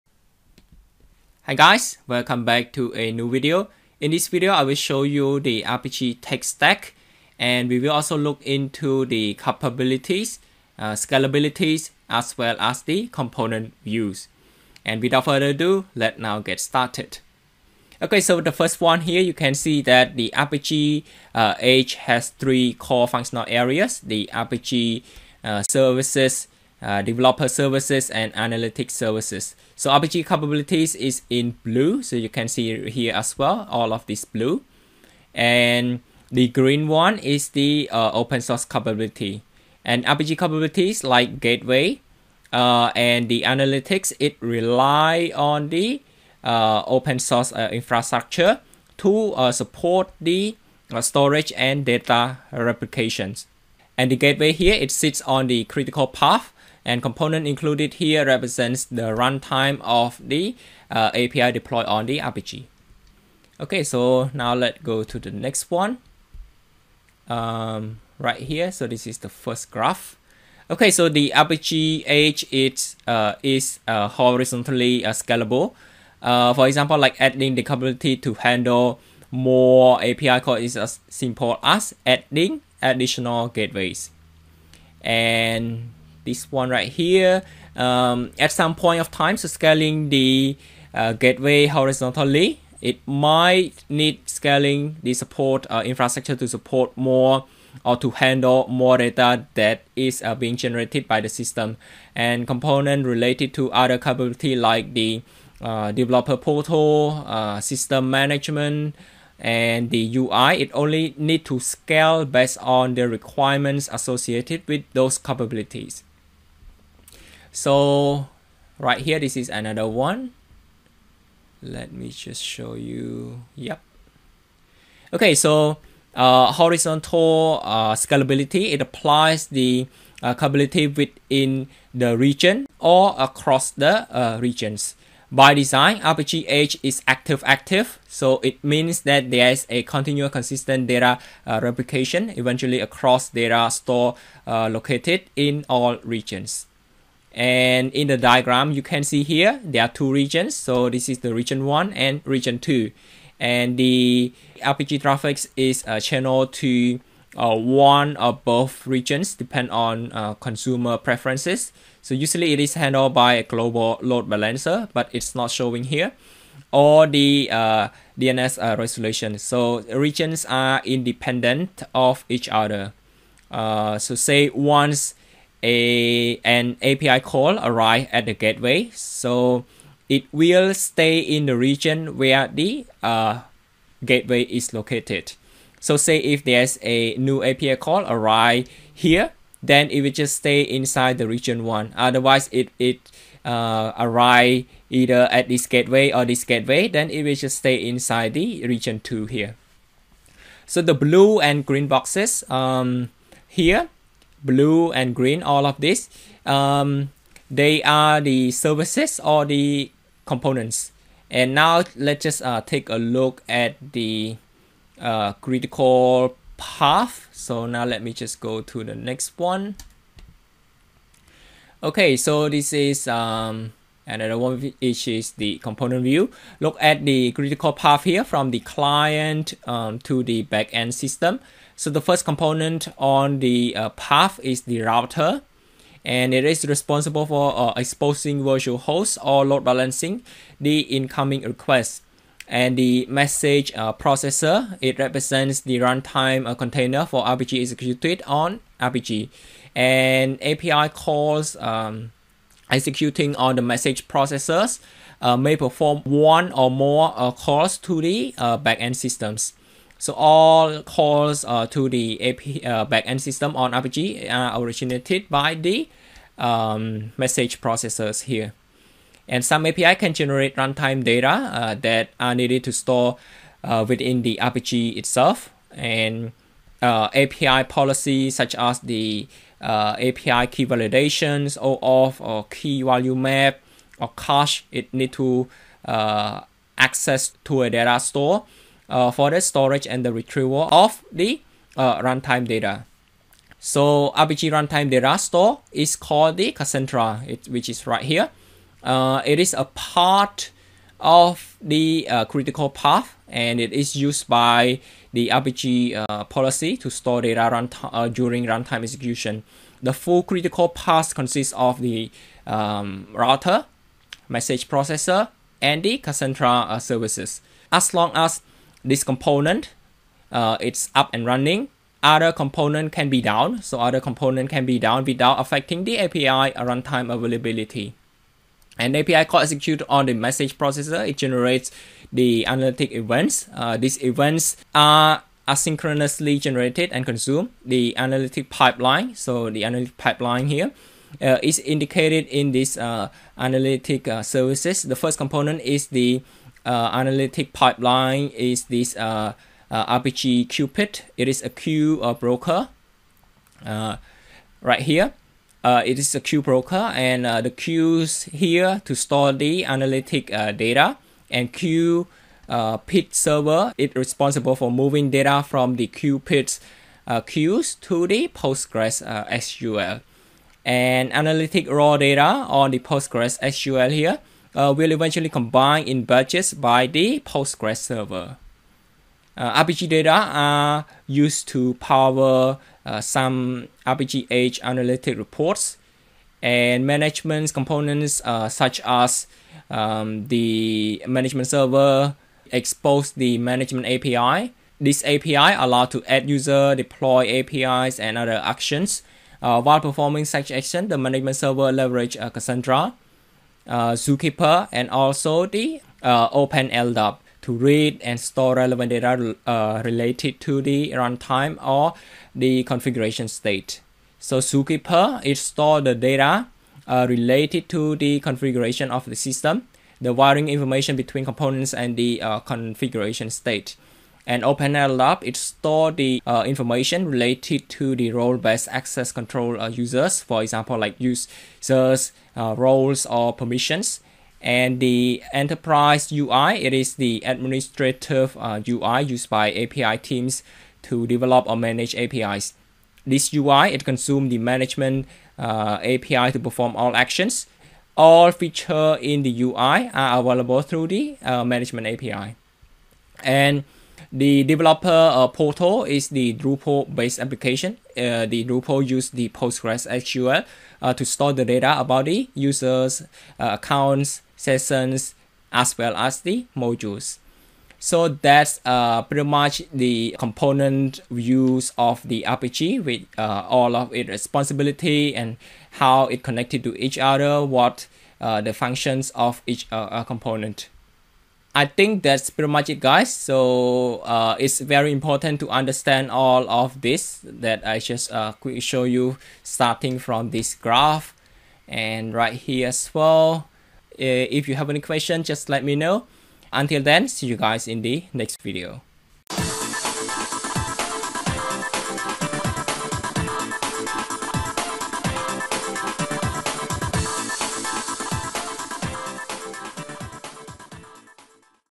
Apigee Tech Stack | Google Cloud Apigee Tutorial
Can you guess what Tech Stack Apigee uses? And let's get to know Apigee's Capabilities, Scalability and Component Views.

⭐ Kite is a free AI-powered coding assistant that will help you code faster and smarter.

🕛 Timestamps:
00:00 Intro
00:18 Capabilities
01:05 Scalability
03:33 Component Views
07:47 Outro